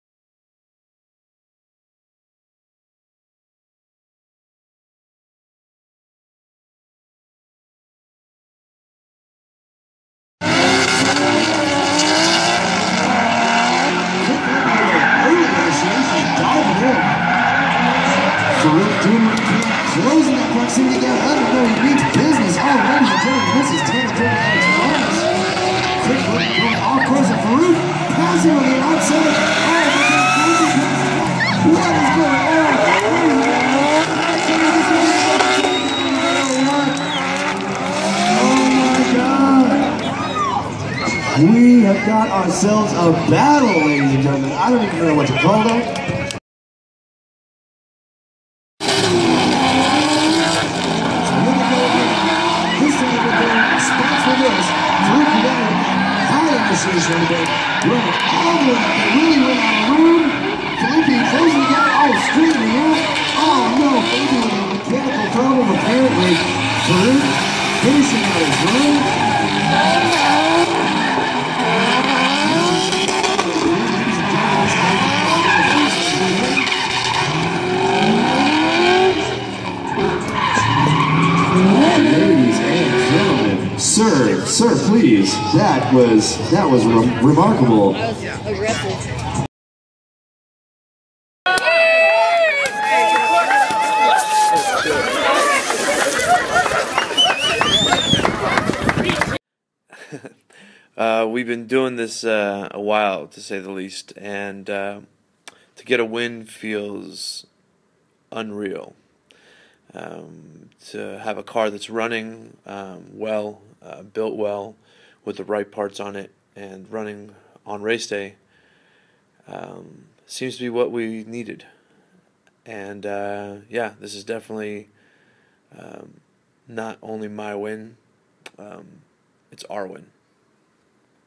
Faruk Kugay - Raze 'til! - Golden Gate Drift Rd2
Faruk took the podium at GGD,  Northern California's Formula D Pro-Am  in Stockton, CA!